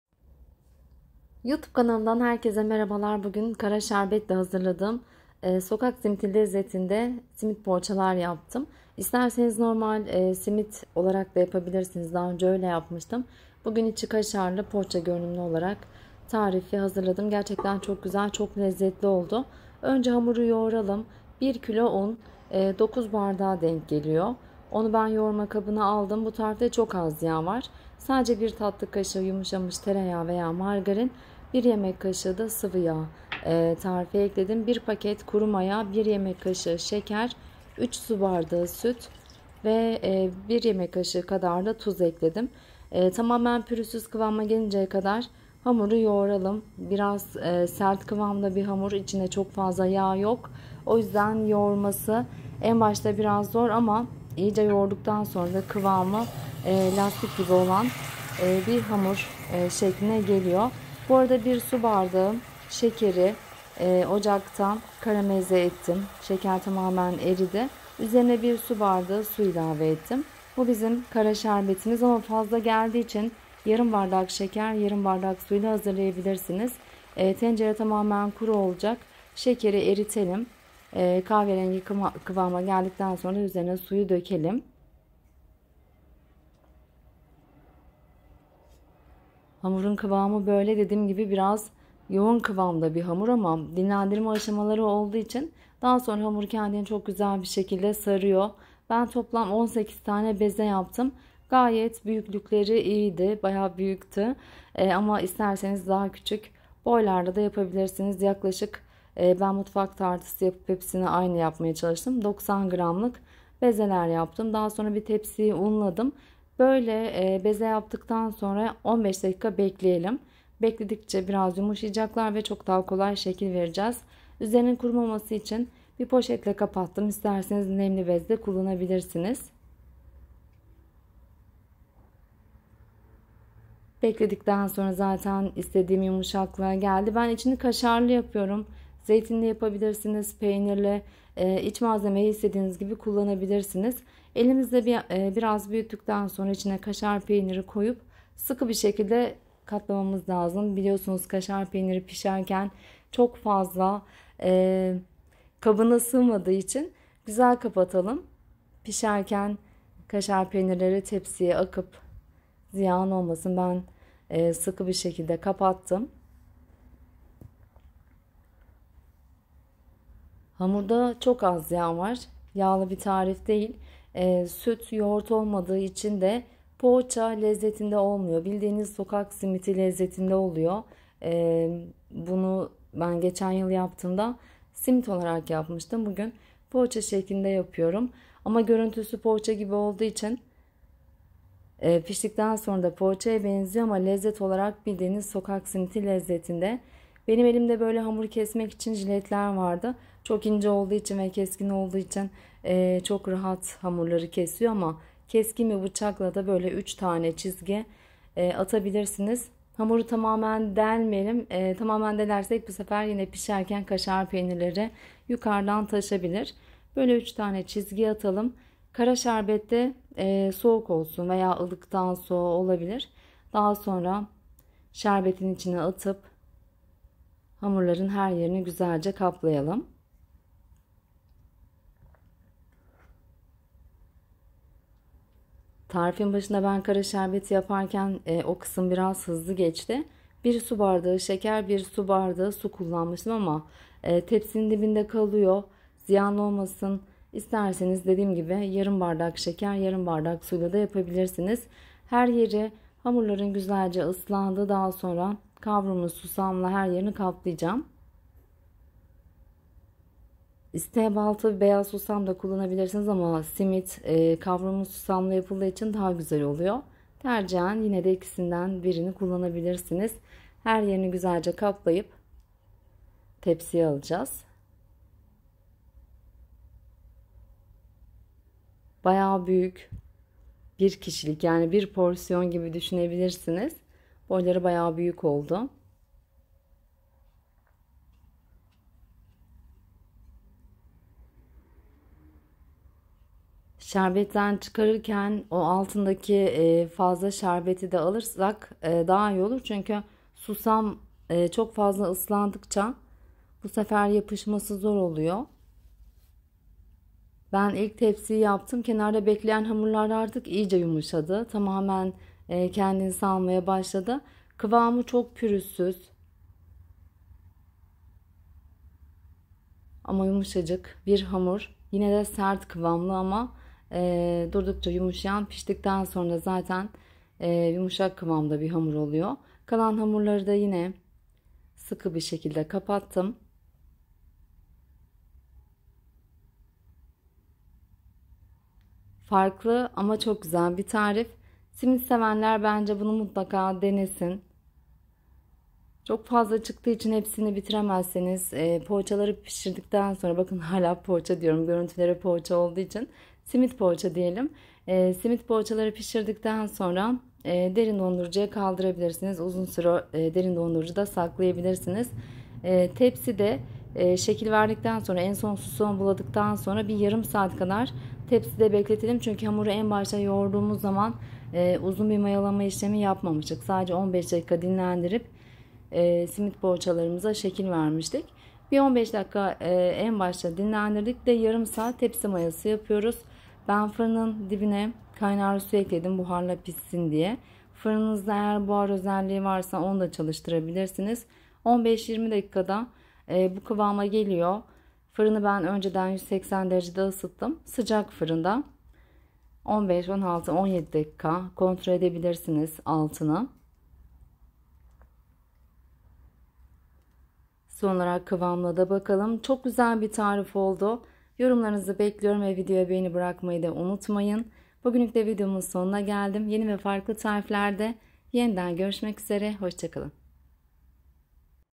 HİÇ BÖYLESİNİ YEDİNİZ Mİ😍 SONUCA İNANAMAYACAKSINIZ, BİREBİR SOKAK SİMİTİ LEZZETİNDE OLDU👌
İnstagram adresim: Lezzeti_ask

Sizden ricam videoya beğeni bırakmayı unutmayın lütfen,
tarifle ilgili yorumlarınızı da bekliyorum🥰

9 su bardağı un( 1 kilo)
3 su bardağı su
1 tatlı kaşığı tereyağ
1 yemek kaşığı sıvıyağ
1 yemek kaşığı şeker
1 yemek kaşığı tuz
1 paket kuru maya(10gr)

Kara şerbeti için:
(Ben tarifi 1 su bardağından hazırladım ama fazla geldi yarım su bardağıyla hazırlayabilirsiniz)

Üzeri için susam

içi için kaşar peyniri